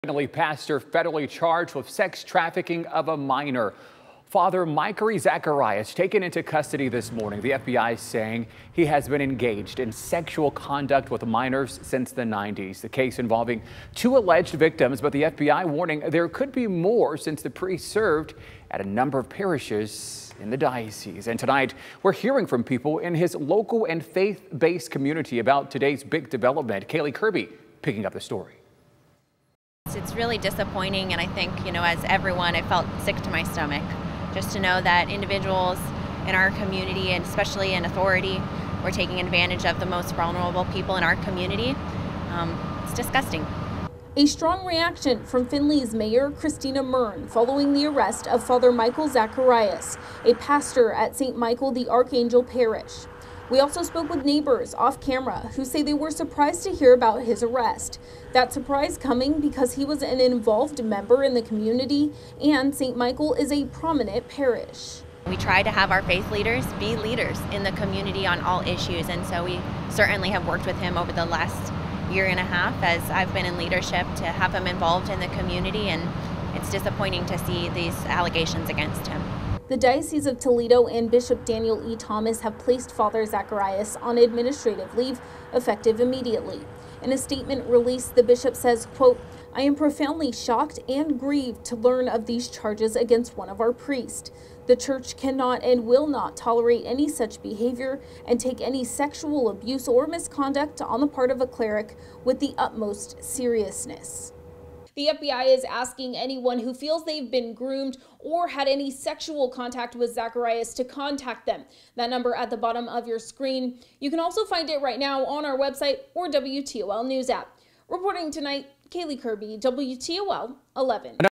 Findlay community reacts to the arrest of Pastor Michael Zacharias
Findlay residents are surprised to hear about the arrest of Pastor Michael Zacharias due to the nature of the charges and how involved he was in the community.